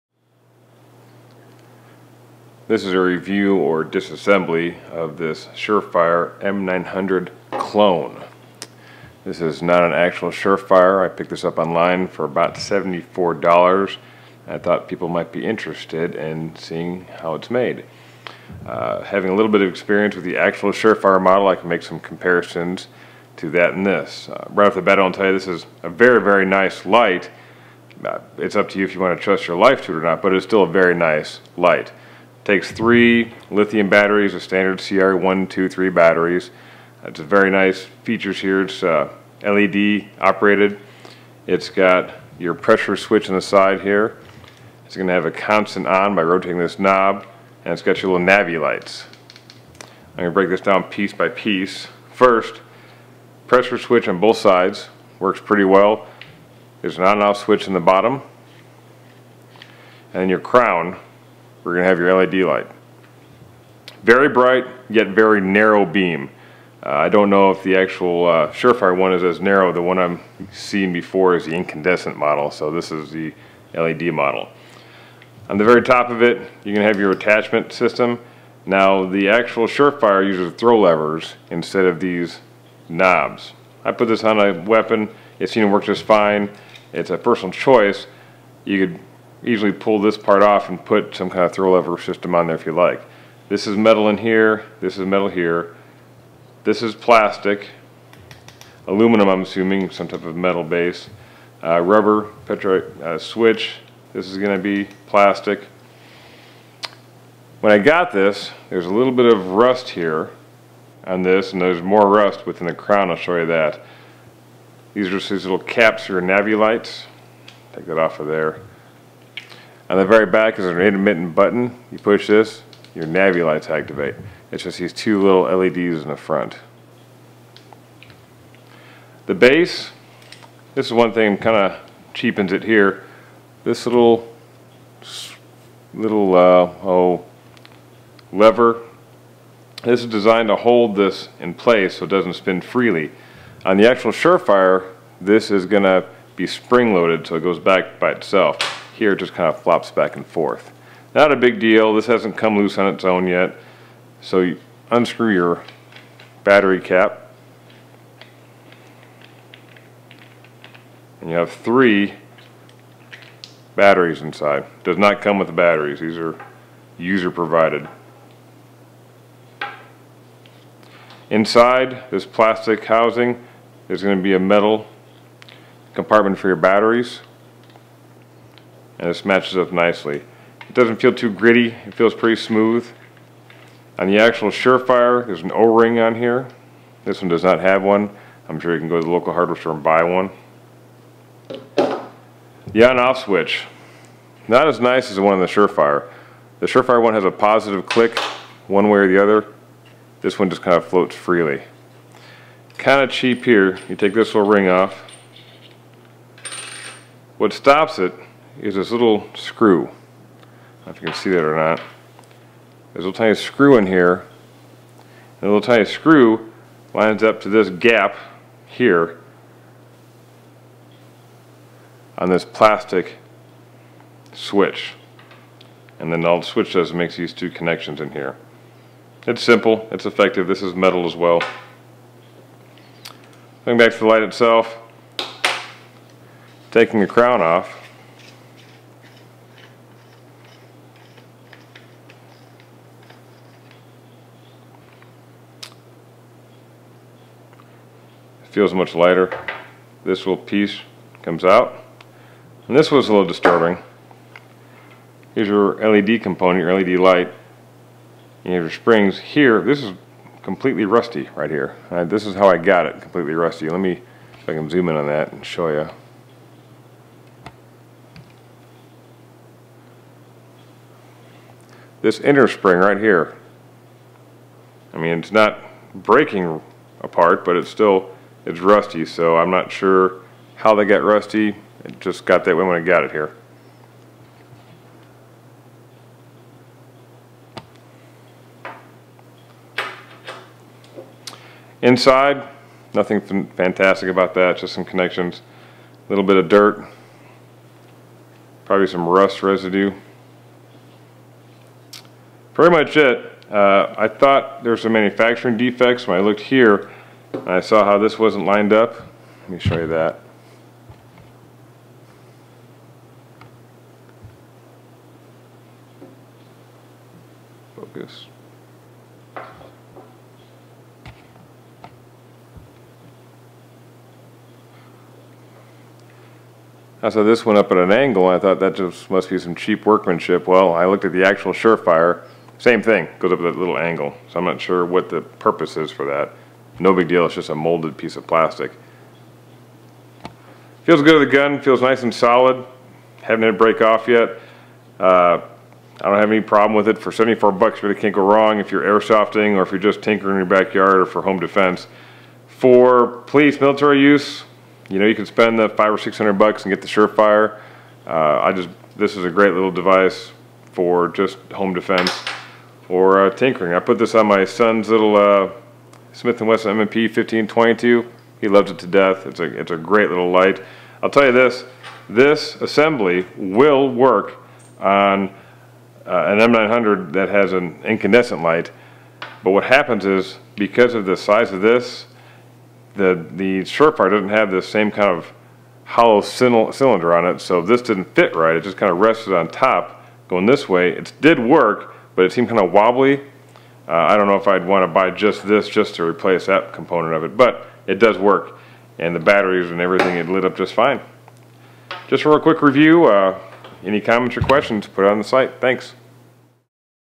Surefire M900 / M910 Clone / Knockoff
Quick review of a clone for the surefire M900 / M910 weapon light. This is a great LED weapon light for the $74 that I paid for it. As this is my first video review, I stumbled a bit through it. Please let me know if you have any questions.

Update - I bought the light through aliexpress.com

Thanks